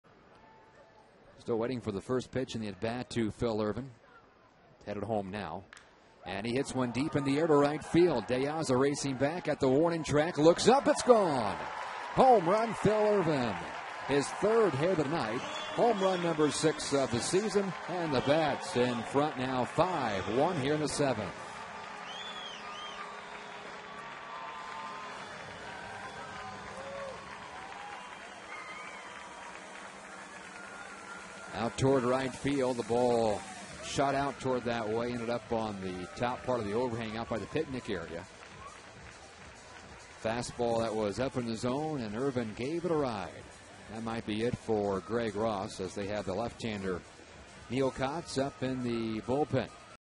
Louisville's Ervin launches his sixth home run
7/18/17: Phillip Ervin's sixth home run highlights his second consecutive three-hit night in Louisville's win over Syracuse.